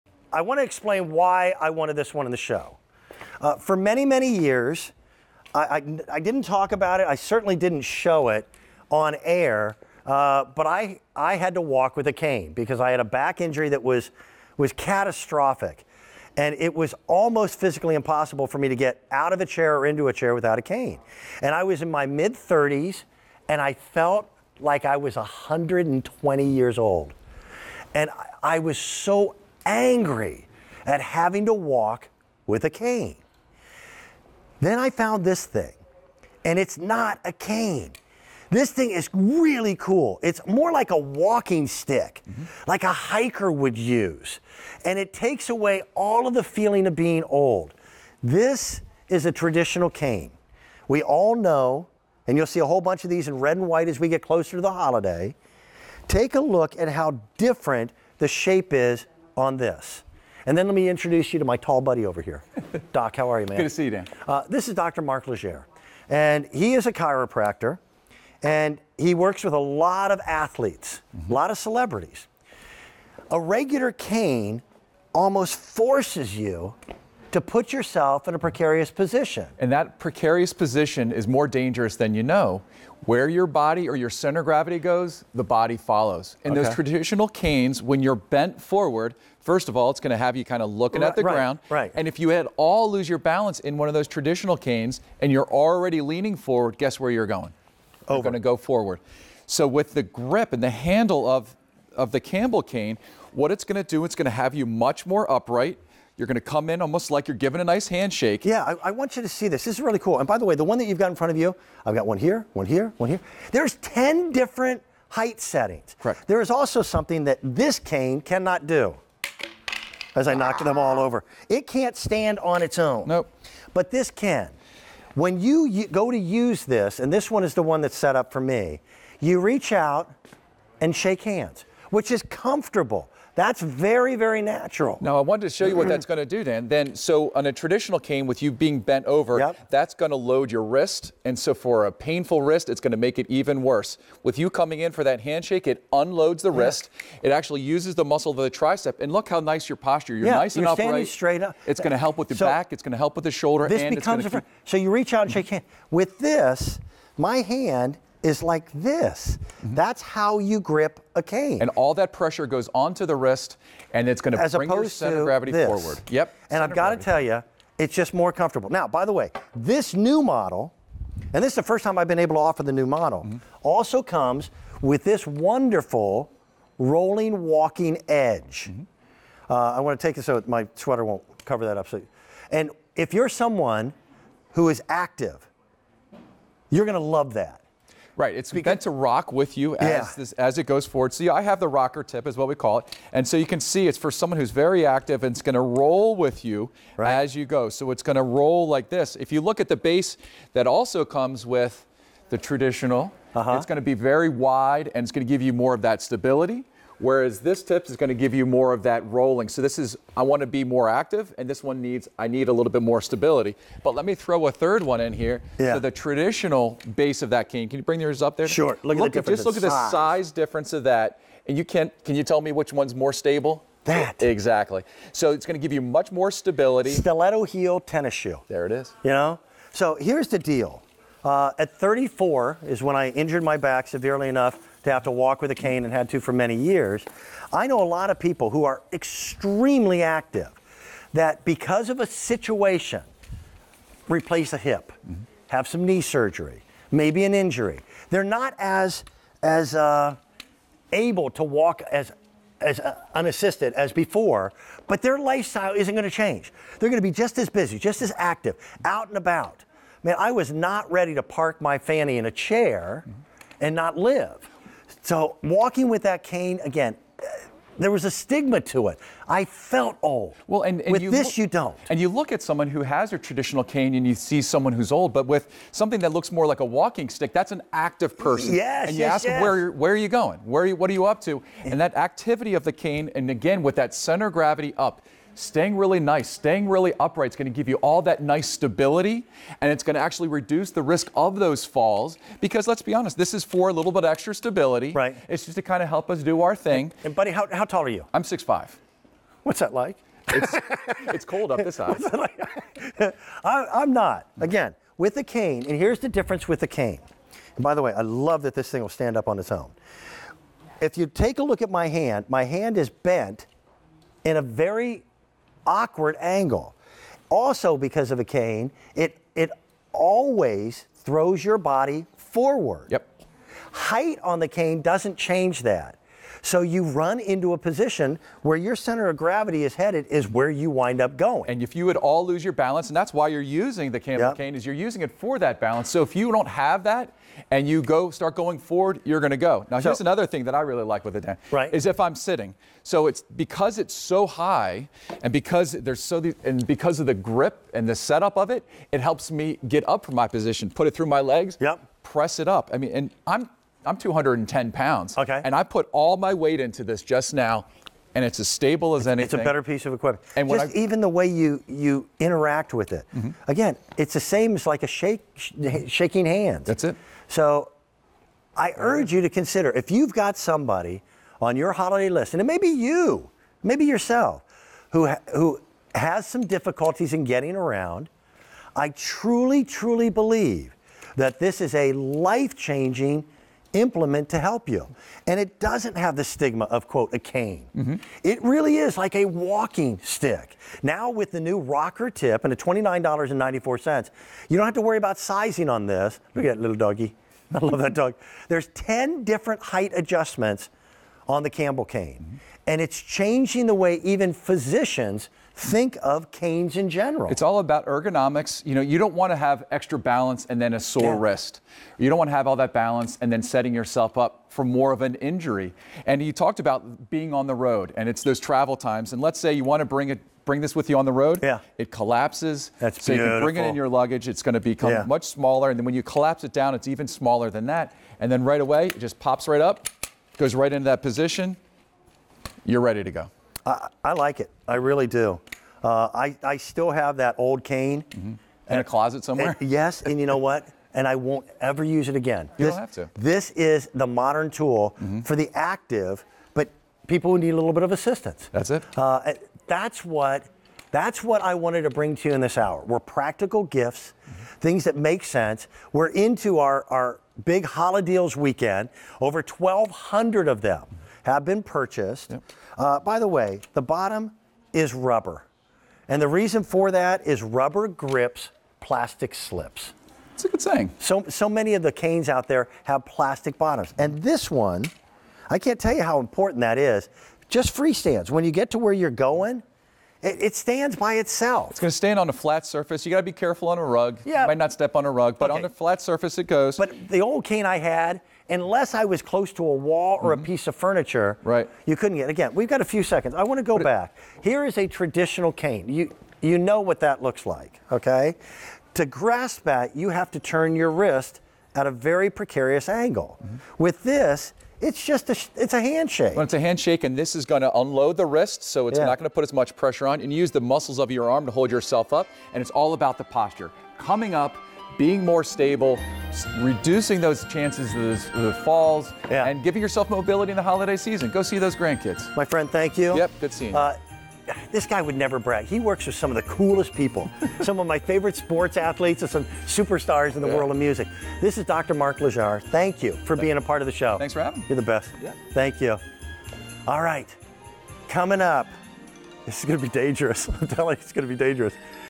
Campbell Cane Adjustable Walking Cane w/ Rocker Tip on QVC
Campbell Cane Adjustable Walking Cane w/ Rocker Tip

Stand up and walk tall -- all while feeling secure while you move. This cane's handle takes pressure off your wrist and shoulder -- making getting up easier. Plus, 360-degree traction helps keep you stable while you're standing and moving. From Campbell Cane.